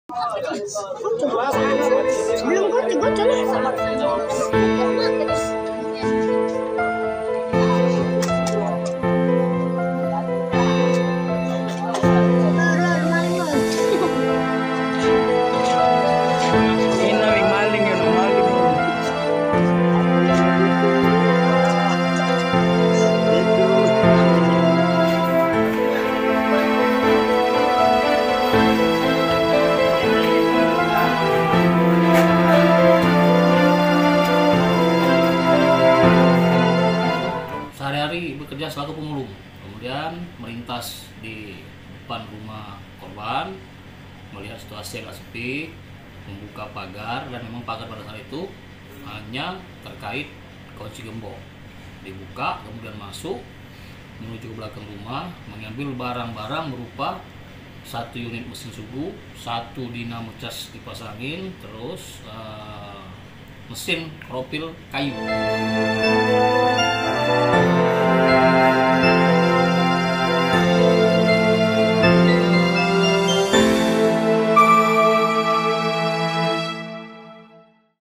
Tertangkap Tangan Mencuri, Pemulung Dihakimi Warga di Palembang
Pria inisial F babak belur dihakimi warga setelah ketahuan mencuri, Rabu (1/2/2023). Kejadian di Jalan Raya Tujuh, Enam Belas Ulu, Seberang Ulu Dua, Palembang, Sumsel. Pelaku sehari-hari bekerja sebagai pemulung di daerah tersebut.

Android: inews.id/android
iPhone: inews.id/ios